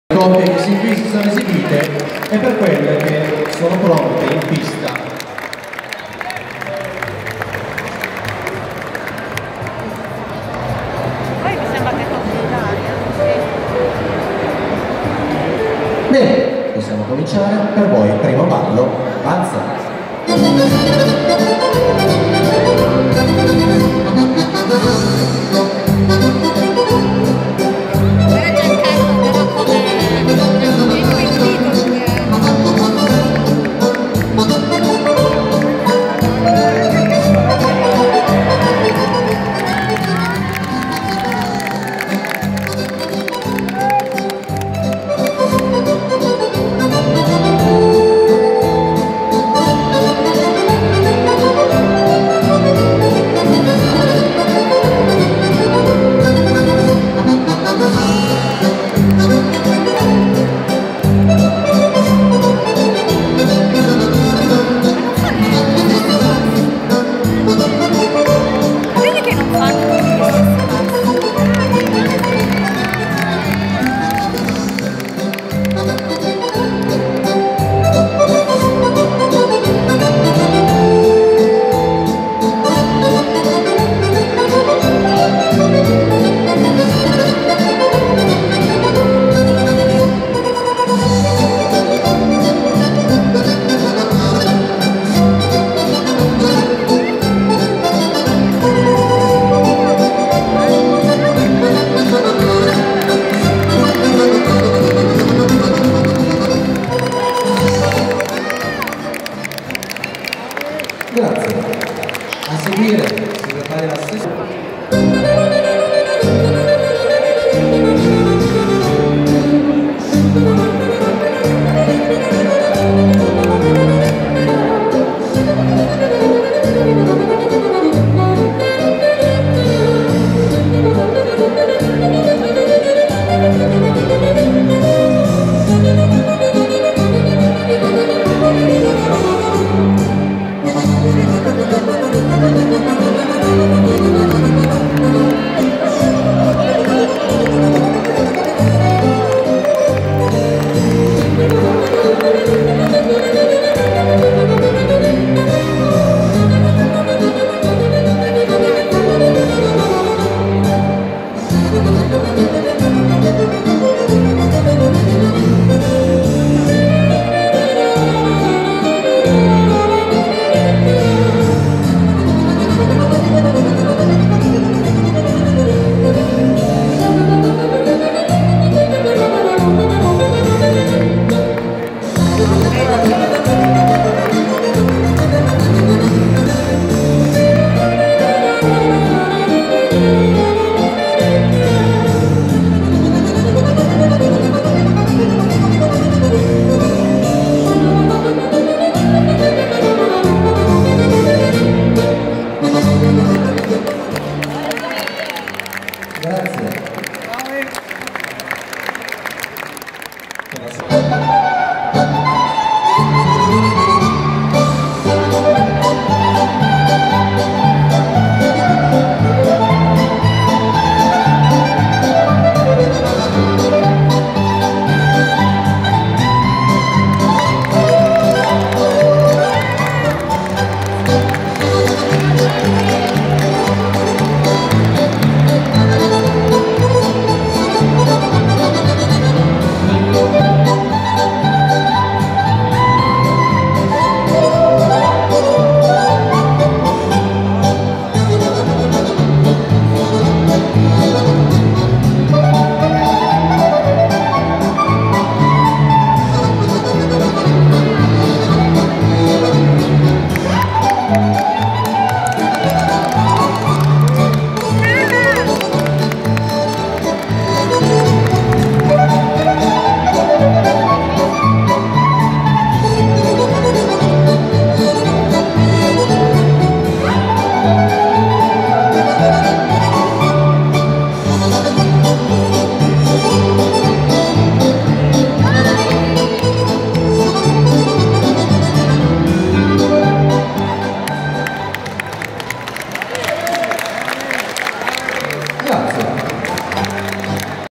Liscio Tradizionale 45/54 - prima gara in B2 Gian e Silvia - Arsa Dance
Liscio Tradizionale 45/54 - B2
Fossano
Gian e Silvia